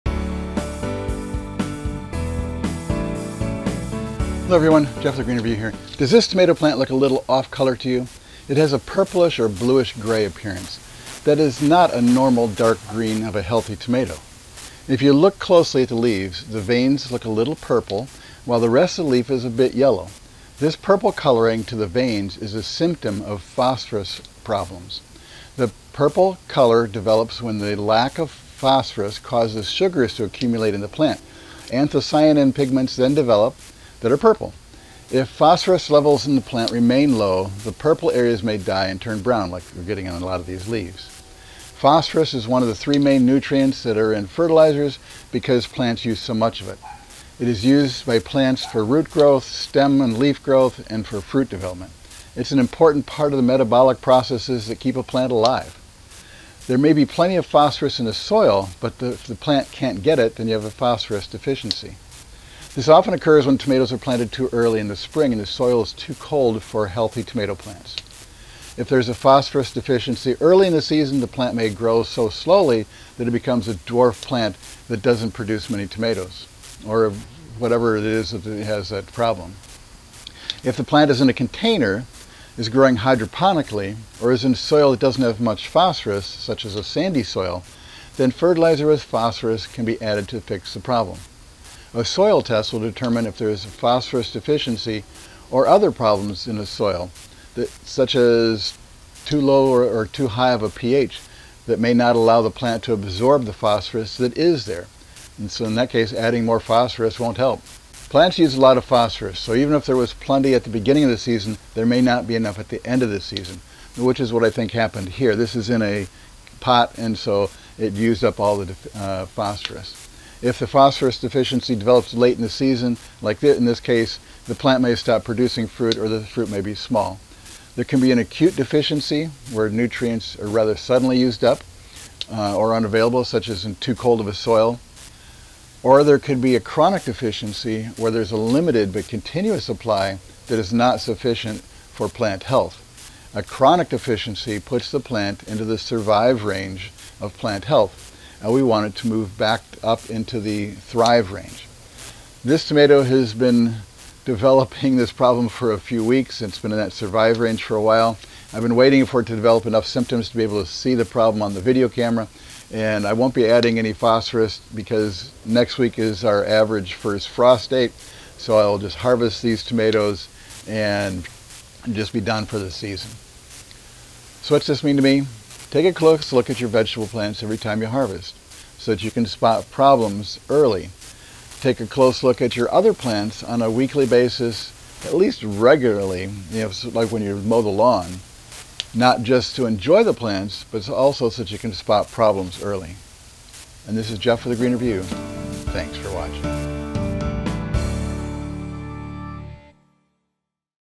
Phosphorous Deficiency
Phosphorus is one of the three main nutrients that are in fertilizers because plants use so much of it. It is used by plants for root growth, stem and leaf growth, and for fruit development. It is an important part of the metabolic processes that keep a plant alive.
There may be plenty of phosphorus in the soil, but the plant can’t get it. This often occurs when tomatoes and other plants are planted too early in the spring and the soil is too cold for healthy plants. If there is a phosphorus deficiency early in the season the plant may grow so slowly that it becomes a dwarf plant that doesn’t produce many tomatoes or other fruit.
Plant use a lot of phosphorus, so even if there was plenty at the beginning of the season, there may not be enough at the end of the season, which is what gardeners see at this time of year. If the phosphorus deficiency develops late in the season the plant may stop producing fruit or the fruit may be small.